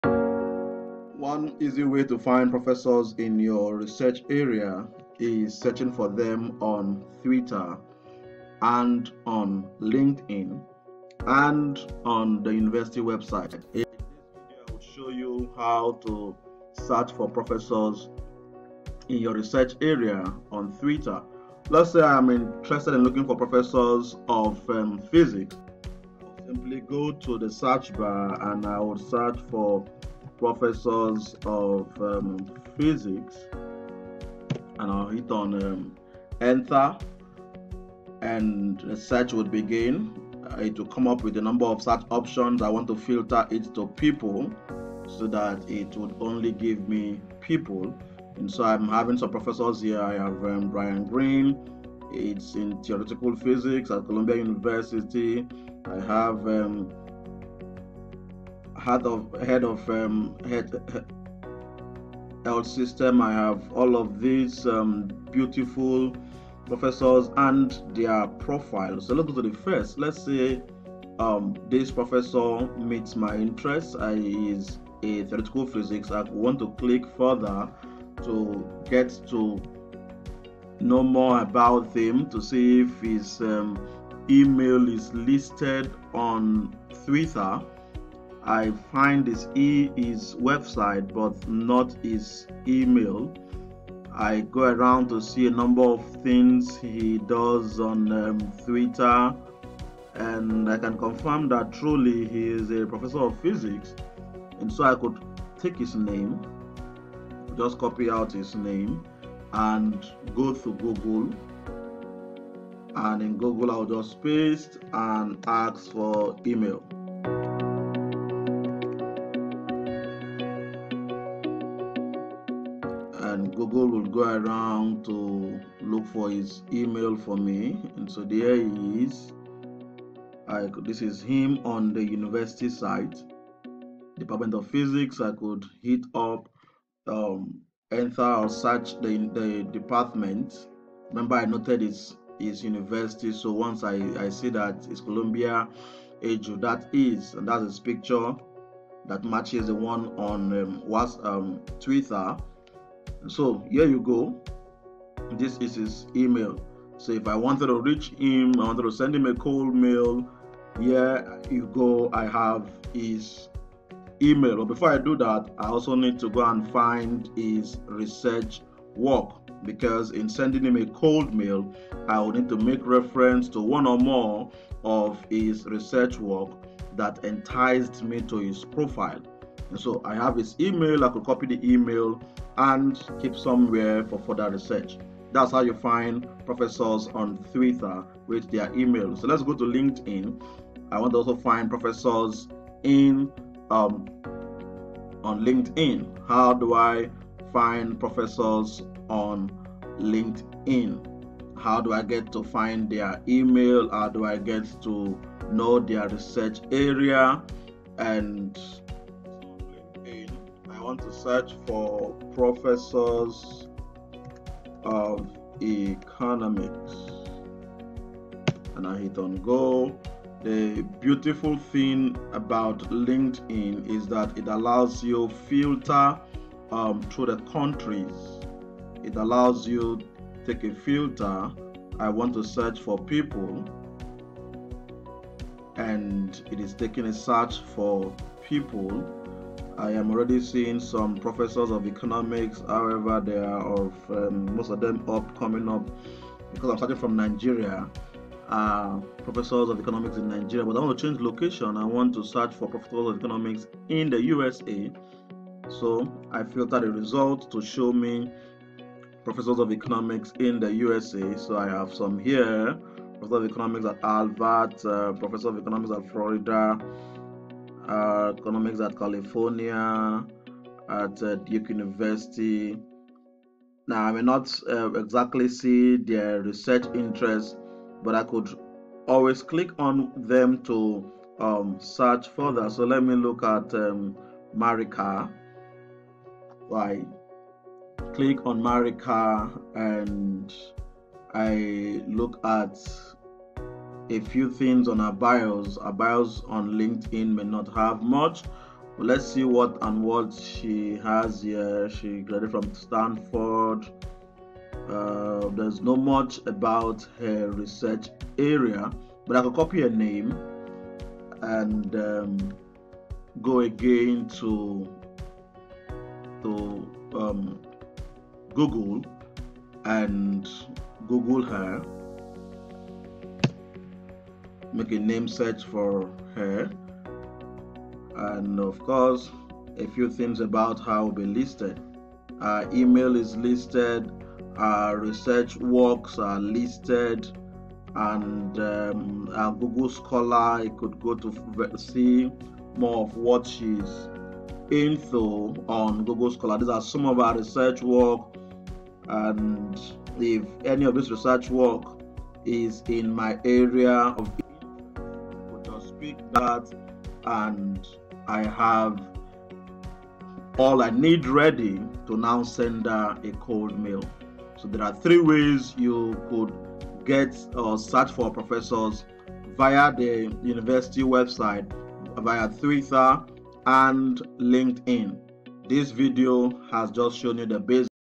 How to search for Professors on Twitter and LinkedIn
You don't know how to search for Professors and PIs in your research area or find their email to send your cold mails? This video explains how to find Professors on Twitter and LinkedIn.